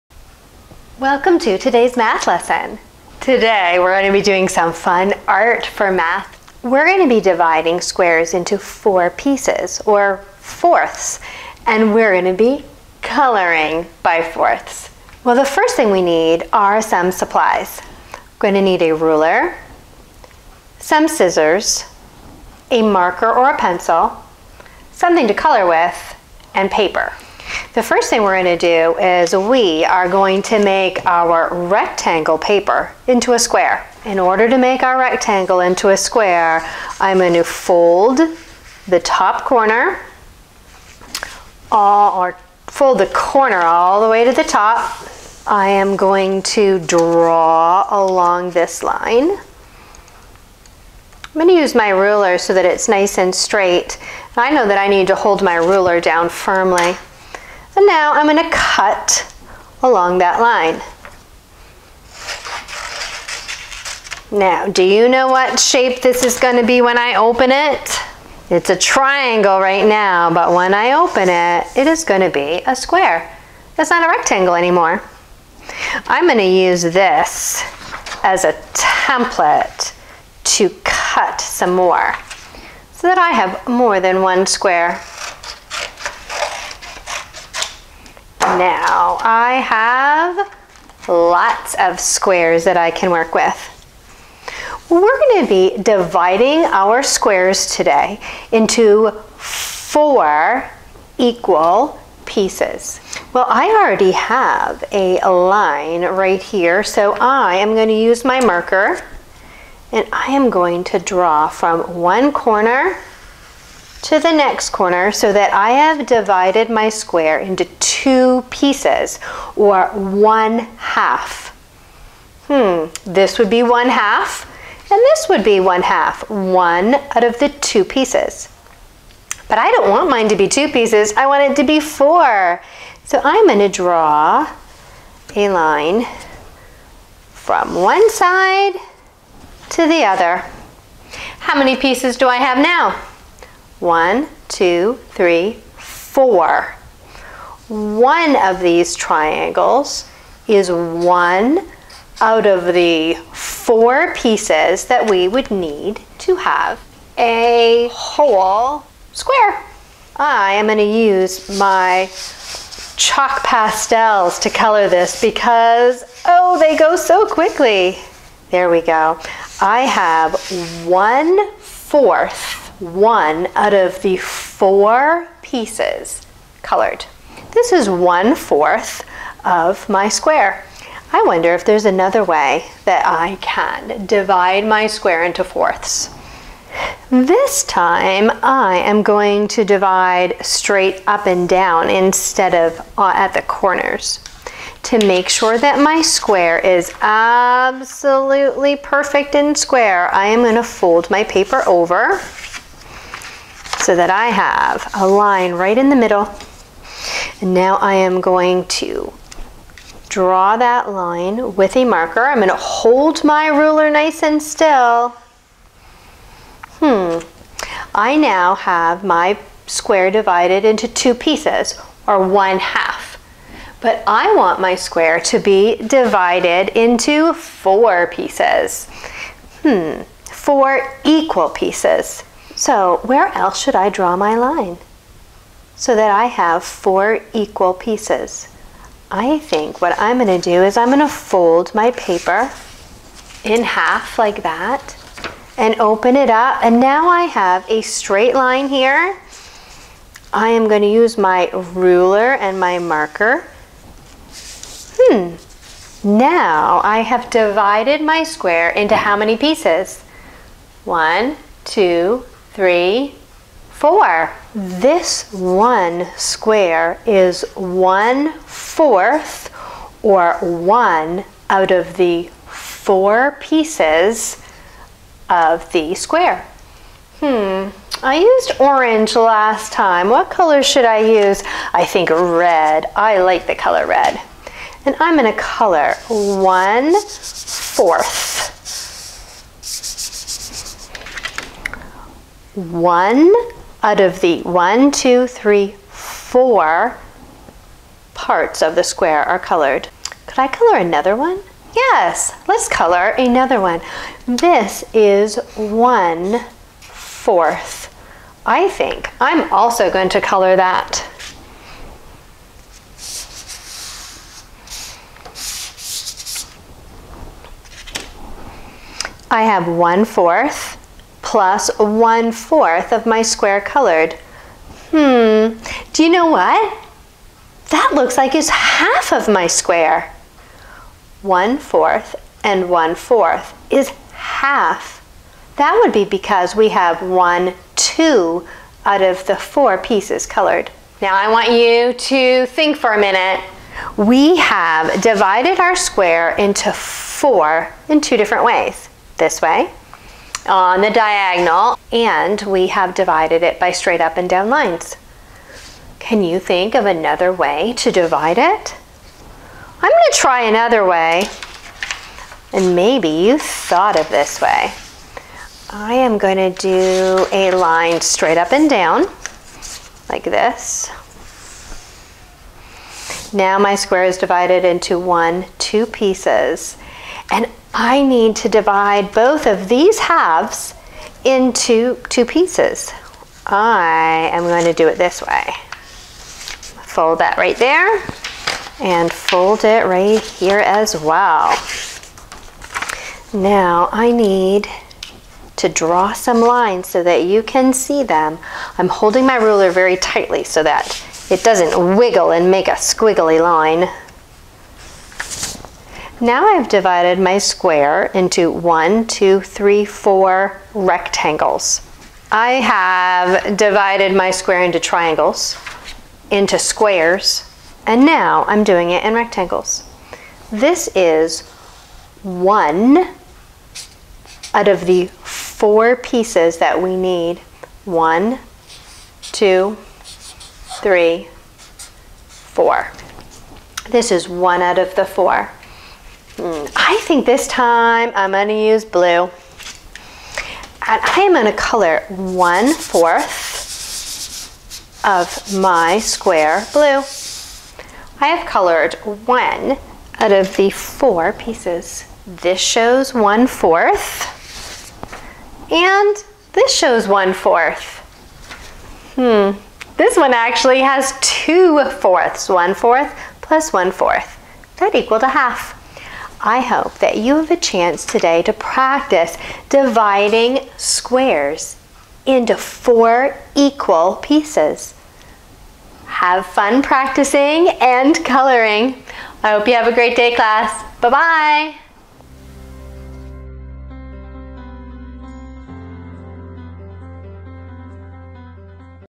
Grade 1: Math Lesson #88 Dividing A Square Into Fourths; Colouring Halves and Fourths
Welcome to our online first grade math class. Join me as I review dividing a square into fourths and colouring halves and fourths.

This video corresponds to the first grade (grade one) Saxon math curriculum lesson #88  Dividing A Square Into Fourths; Colouring Halves and Fourths.   It also introduces concepts included in the first grade math curriculum from Abeka, ACSI, Scott Foresman,  Harcourt Math, and Math You See.